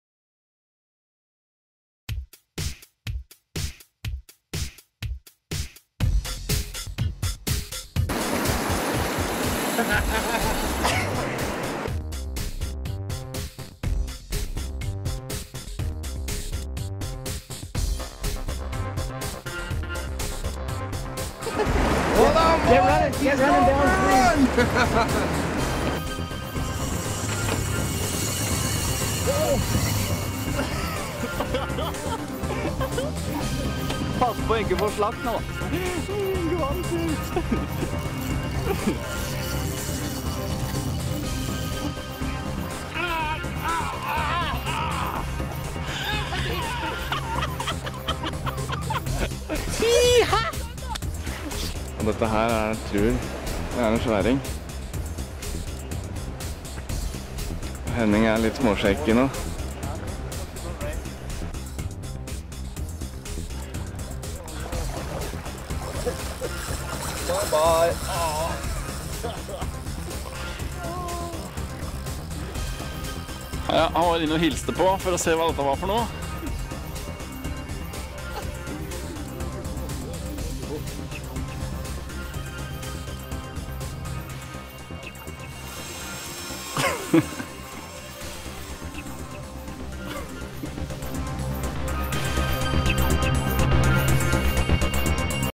Chinook Salmon - Kongelaks - Königslachse - Trailer
The greatest salmon fishing in the world is waiting for you in Terrace British Columbia, Canada. This small city situated on the banks of the Skeena River at the heart of Skeena region not only offers the largest king salmon in B.C. but Coho salmon, sockeye salmon, pink salmon and chum salmon as well. Dolly Varden char, bull trout, and sea run cutthroat trout are caught frequently in the main stem of the Skeena and in the many tributary rivers only a short distance away.